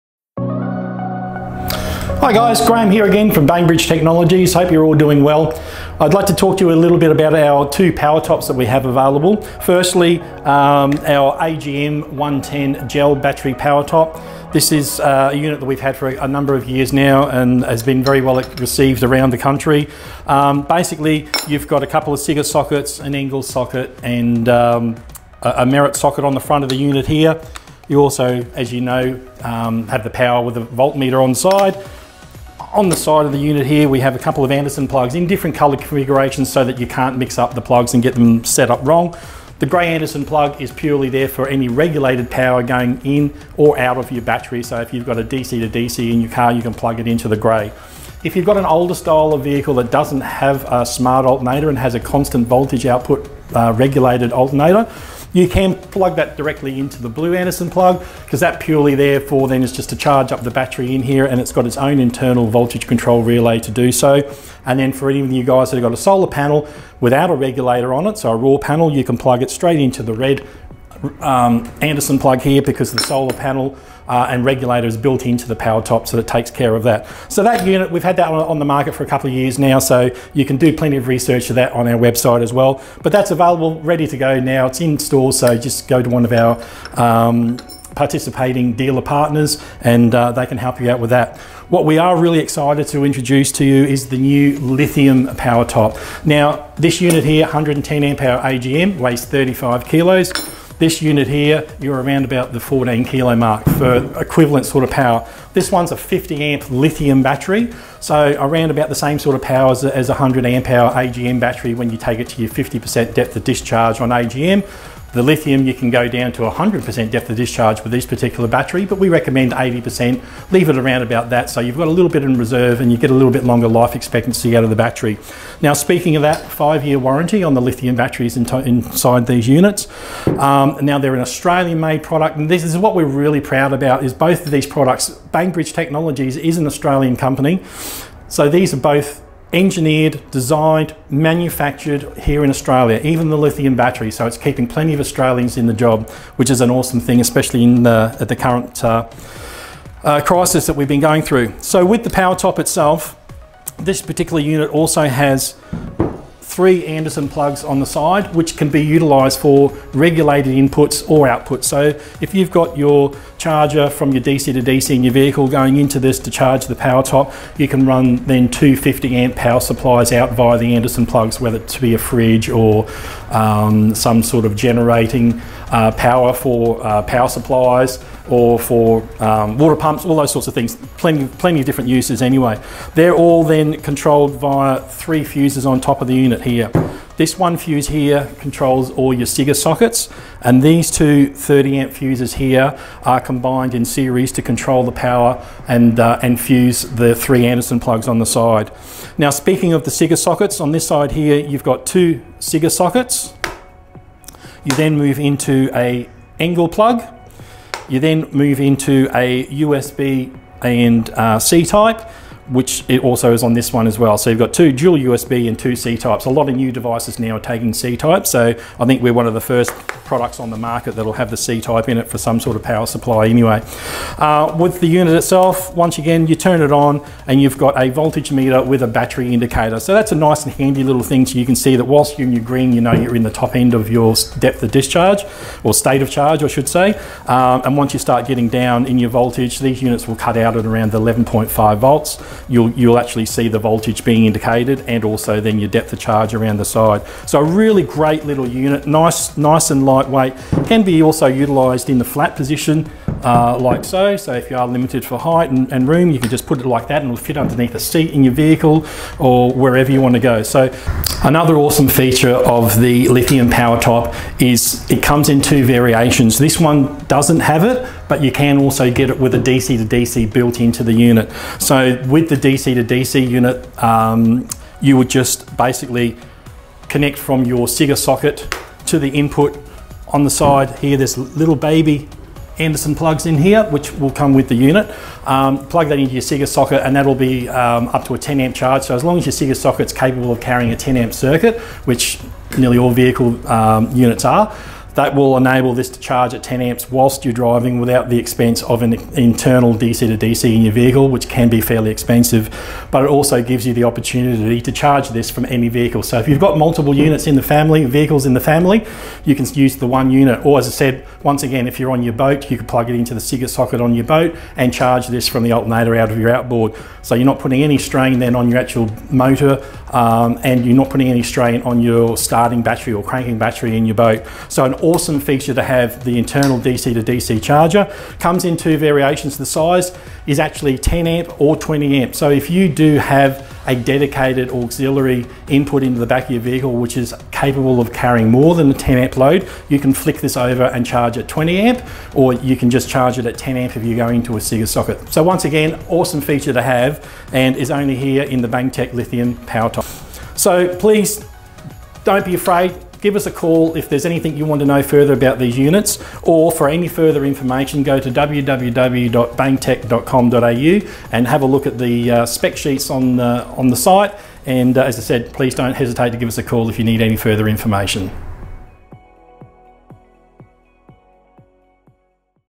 How To : Baintech Lithium Dual Battery Set Up
The Baintech dual battery set-up with a difference. The Baintech Lithium 50Ah PowerTop is a standalone portable power product that can be removed from your vehicle, taken on your boat, set up at camp, or mounted in your caravan.

Keeping your dual battery system portable means your battery is not tied to one vehicle or application.

Why Lithium?

Lithium batteries provide more usable capacity than lead acid batteries. Deep discharging does not affect the life of a lithium battery the way it does with lead acid batteries. You can use the battery longer before needing to recharge, without damage to the battery.

Cost Effective Over Life Cycle
When considering the usable capacity and lifespan of lithium batteries, they work out cheaper than lead acid. When you add the cost and inconvenience of refitting new lead-based batteries every few years the total cost of ownership over the long term is far better with lithium batteries.

Longer life
Lithium batteries are rated for 2,000 – 5,000 cycles and much more if used carefully, giving you several times the life expectancy of standard lead acid batteries.